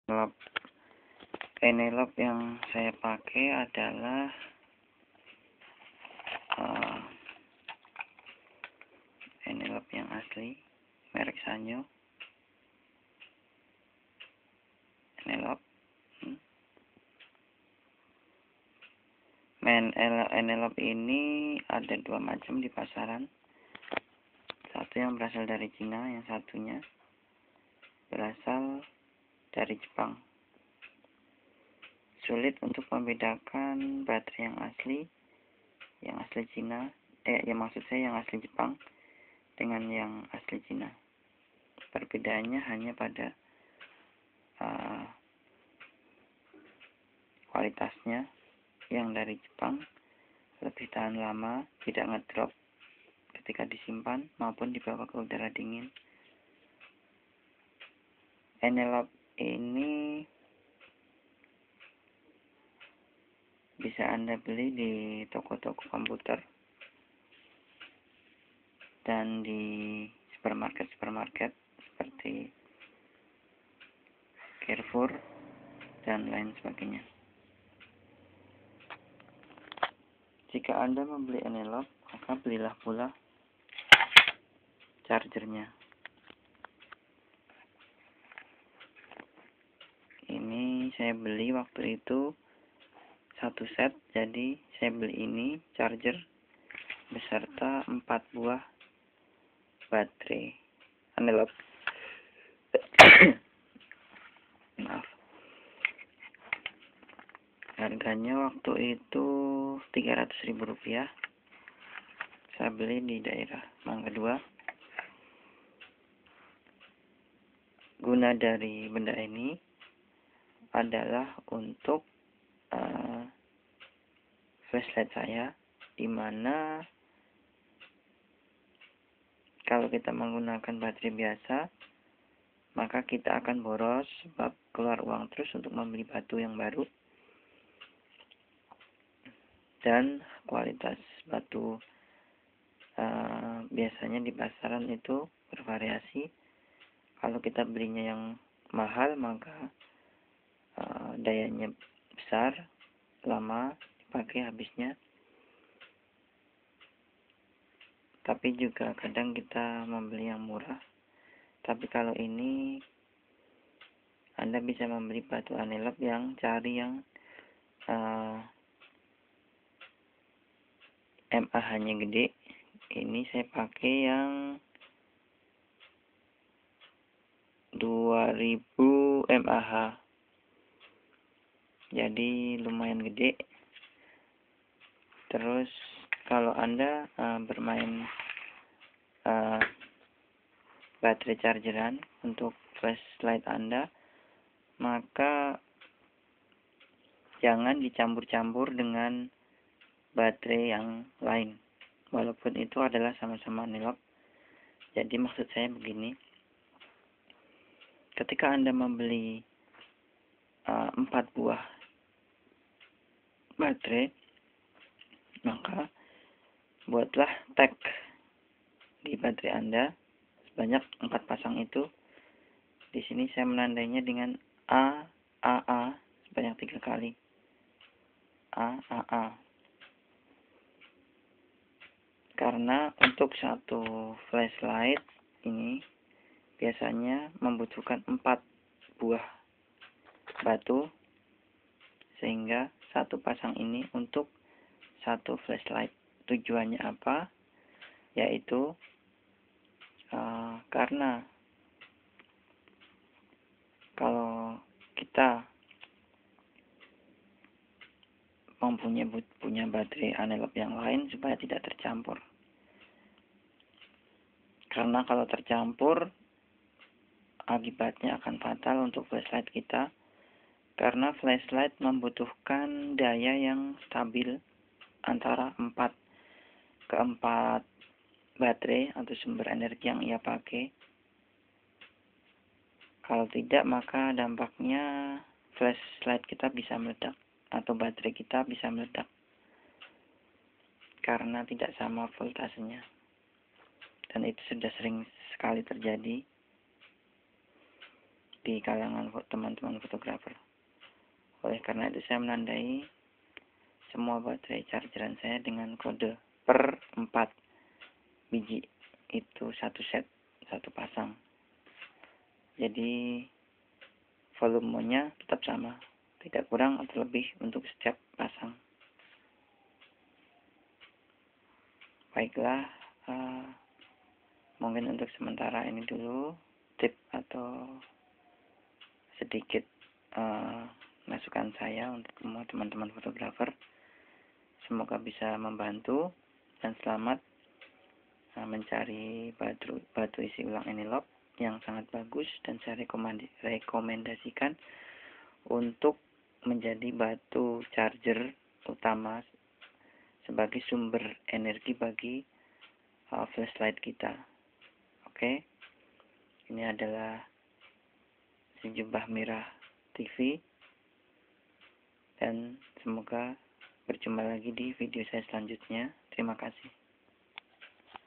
Baterai anelop part 2.3gp
Batu baterai isi ulang yang sangat diekomendasikan untuk flash light dalam fotografi